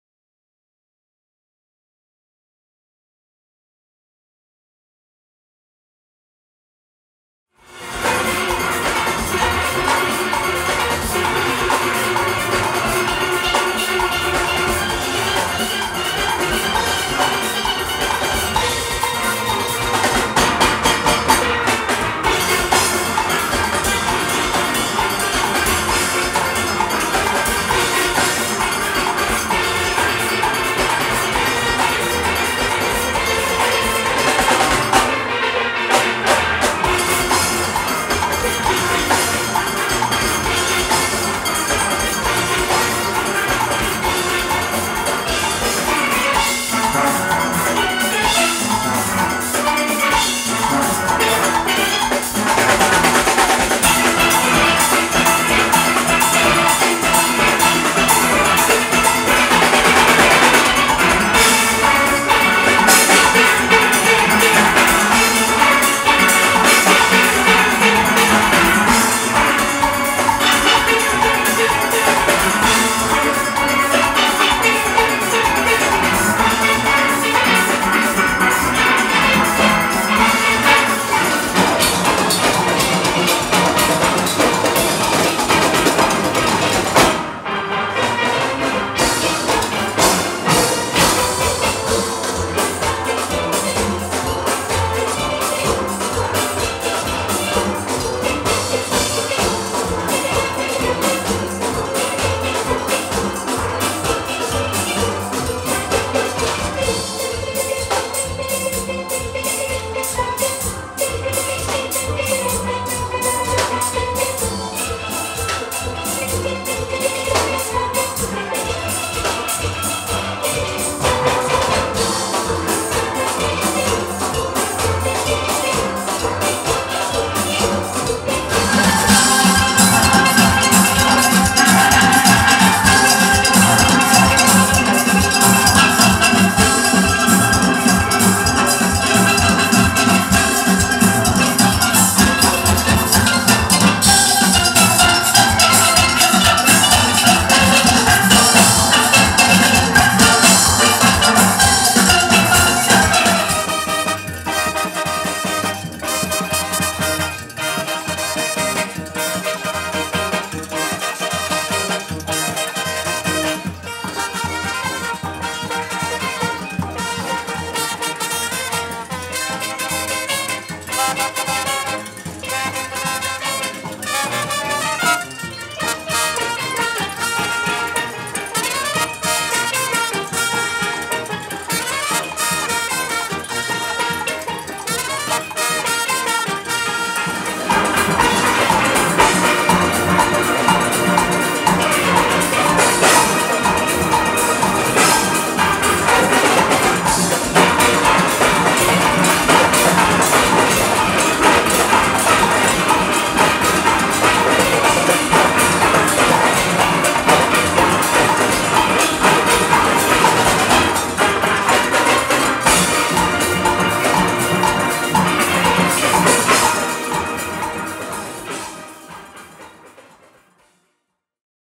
Pan Pun De Sand 2014
Pan Pun De Sand 2014 featuring: Massy All Stars, Mosaic, Olakunde Steel Orchestra, Play As You Go, Massy All Stars & Mosaic.